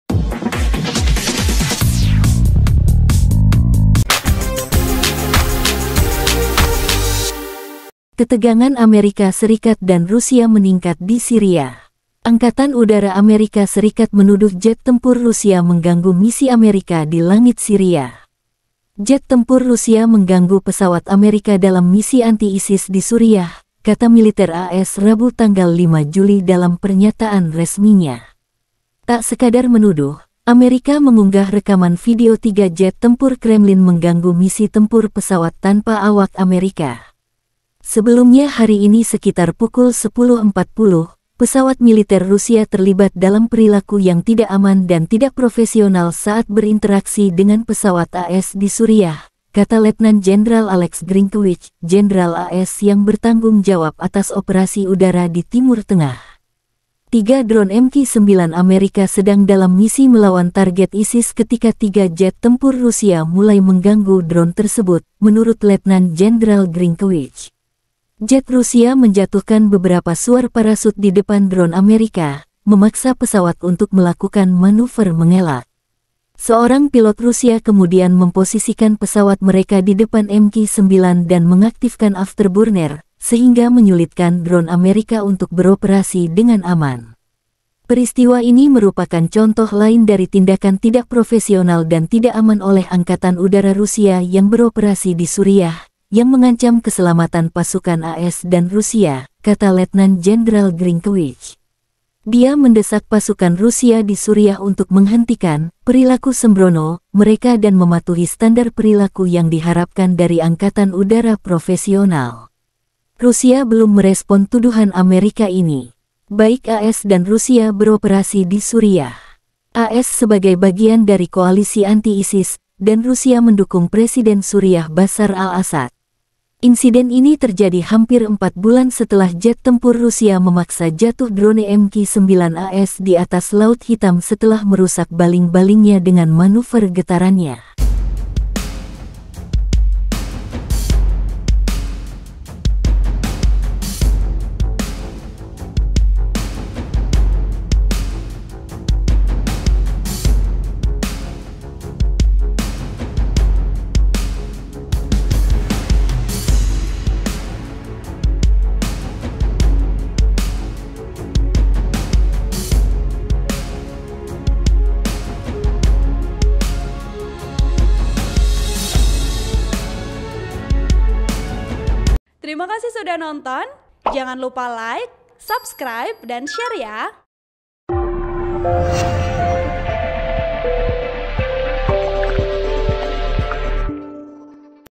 🔴 Amerika vs Rusia Memanas! Jet Tempur Su-35 Rusia 'Main-main' dengan MQ-9 Reaper AS
TRIBUN-TIMUR.COM - Ketegangan Amerika serikat dan Rusia meningkat di Syria.

Angkatan Udara Amerika serikat menuduh jet tempur Rusia mengganggu misi amerika di langit siria.

Jet tempur Rusia mengganggu pesawat Amerika dalam misi anti-ISIS di Suriah, kata militer AS Rabu 5 Juli.
Tak sekadar menuduh,  Amerika mengunggah rekaman video 3 jet tempur kremlin mengganggu misi tempur pesawat tanpa awak Amerika.(*)